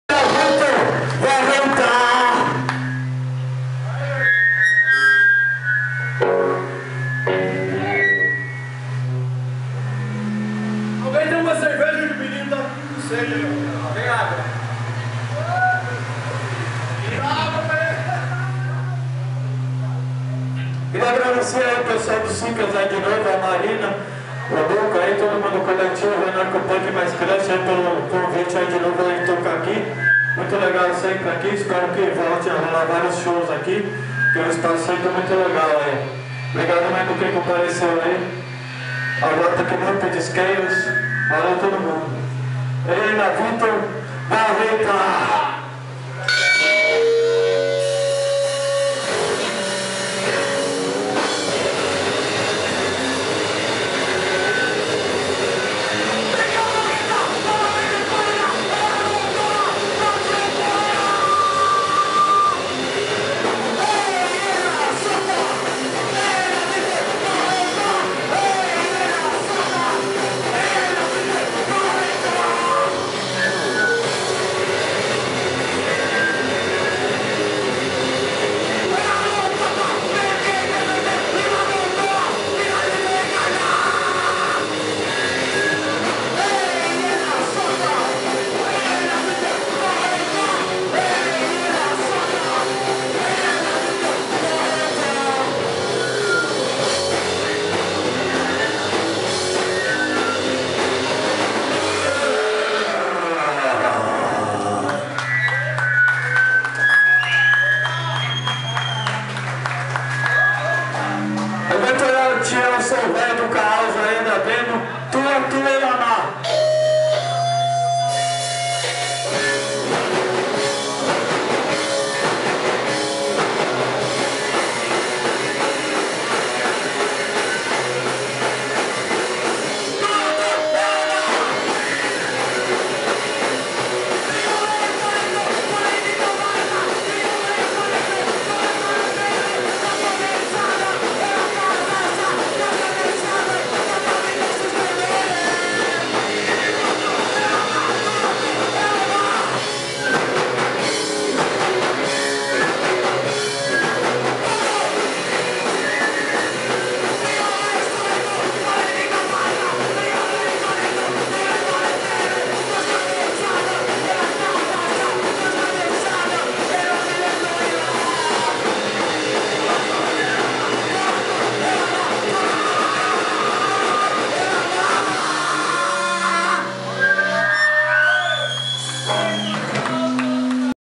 Helvetin Viemärit @Cicas, SSão Paulo, $P 26.03.2016
Hardcore/Punk finlandês feito em São Paulo ao vivo no dia de Cultura Punk IV no Cicas.